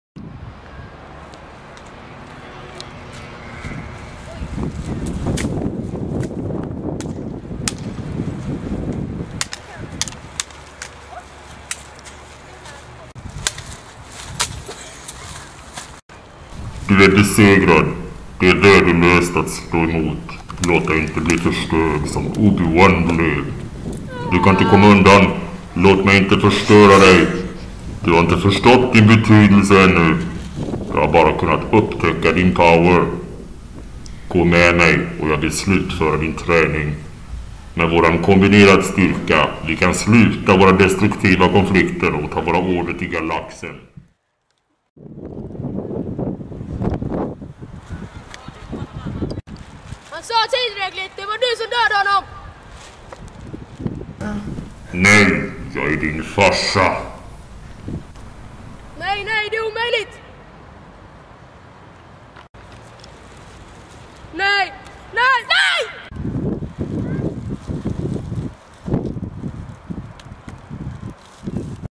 Skapande skola Kungsbacka VT 2015 åk 6 Hålabäcksskolan Star Wars #1
Här kan ni se filmer av eleverna i åk 6 Hålabäcksskolan i Kungsbacka

Hollywood goes short

Ett projekt i Skapande skola Kungsbacka VT 2015.

Projektet Digital Produktion -- lustfyllt lärande
Genom att arbeta med estetiska lärprocesser vill vi ge eleverna möjlighet att skapa något med modern teknik och webbpublicering i kombination.